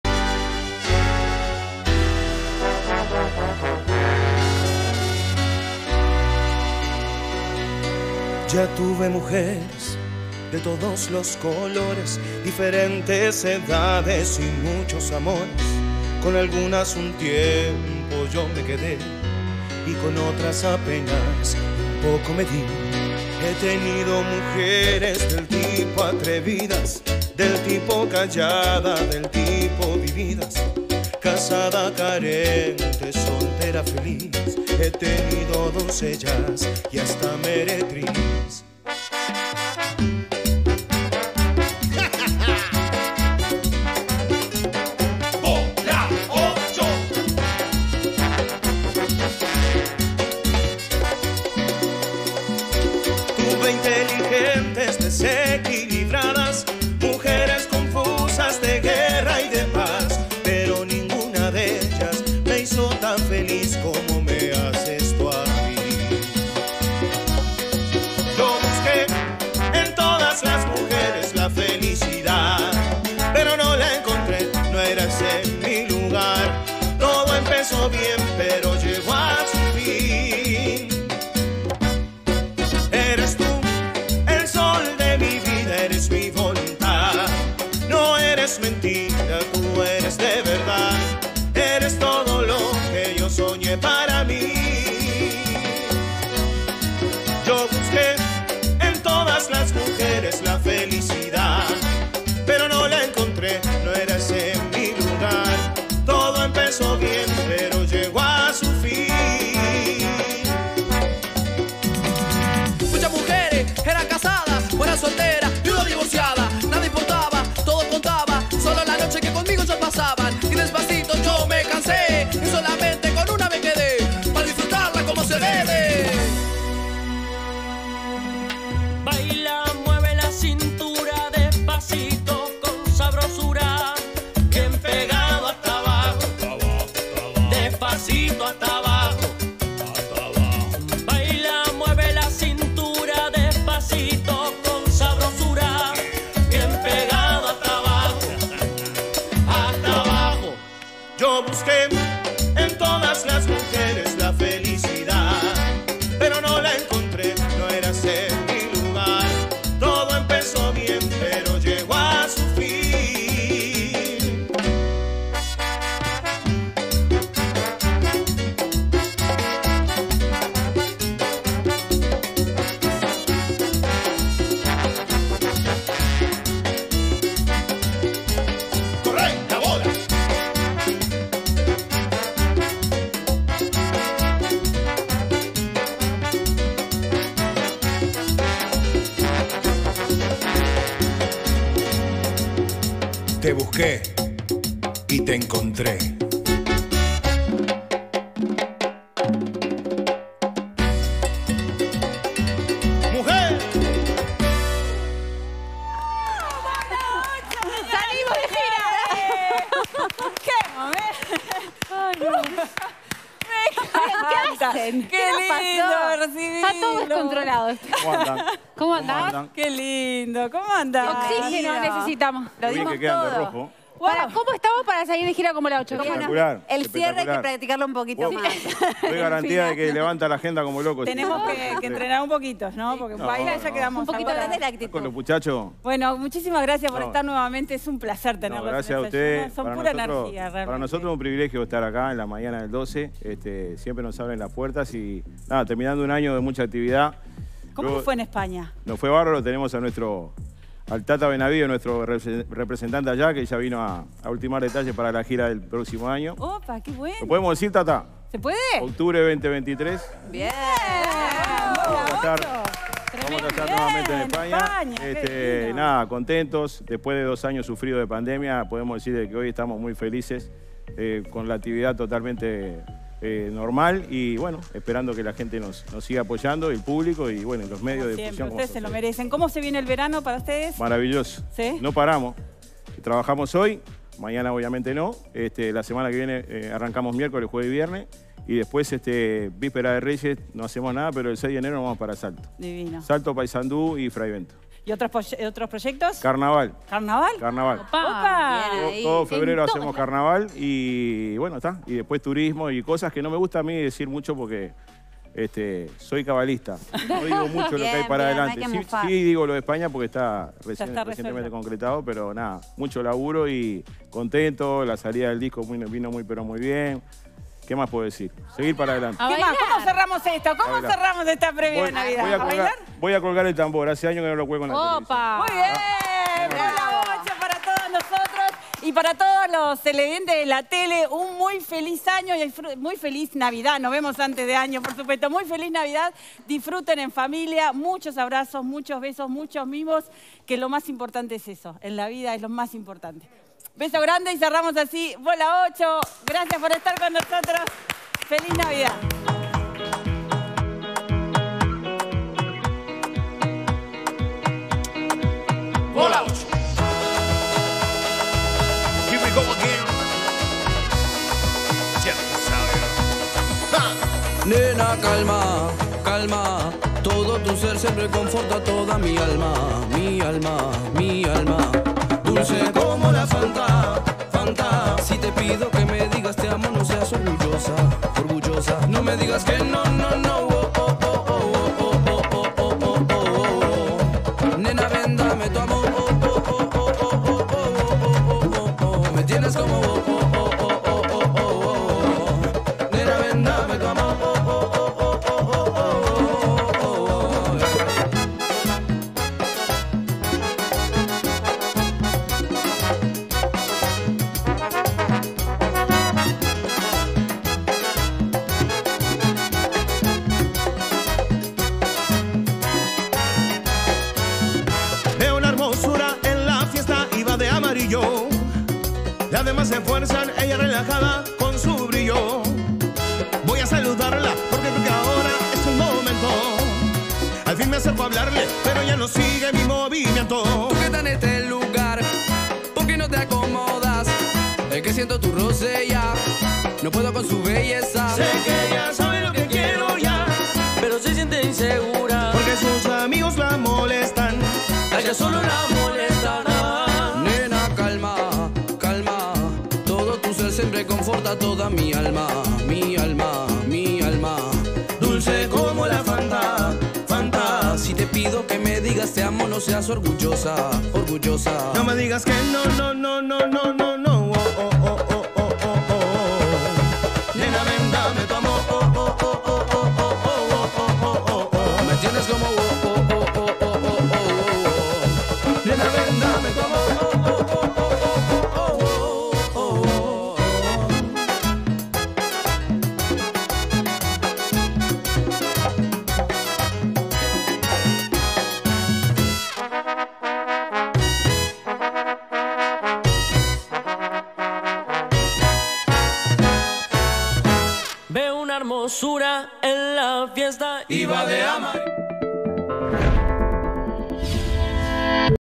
Bola 8 nos cuenta sobre su gira por España y sus próximos pasos
La banda uruguaya Bola 8, que se creó en el 2001, hizo una gira por España este año y nos cuentan sobre sus proyectos para el año que viene.